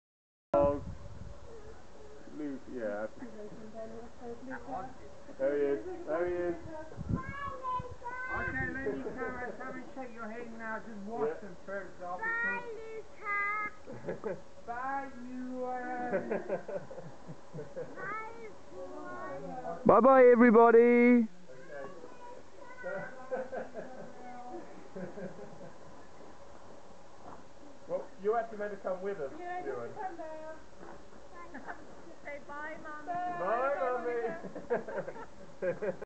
Bye, Bye Fritzy! (time to leave La Faula!)
Leaving La Faula to go home often requires special time and hugs with the dogs!